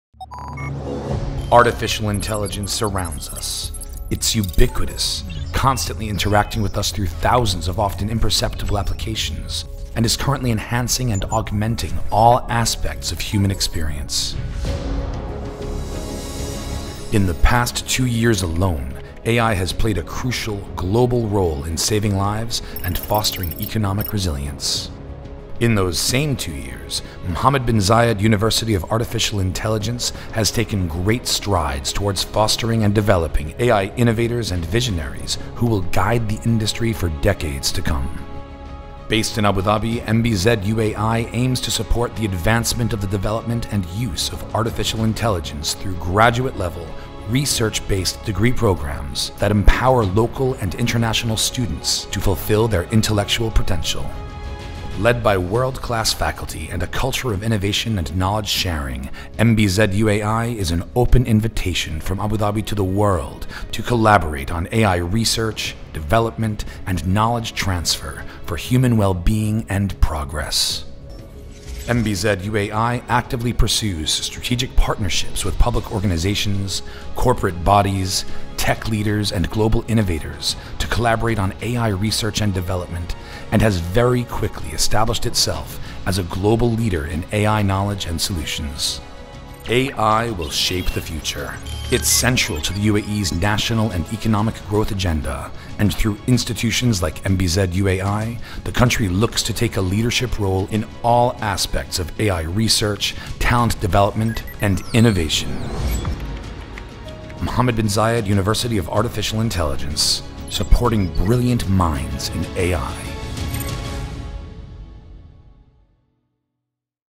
MBZUAI - Empowering brilliant minds in AI
The Mohamed Bin Zayed University of Artificial intelligence is an open invitation to the world - to make Abu Dhabi your launchpad - as you unlock the potential of AI as a force for good.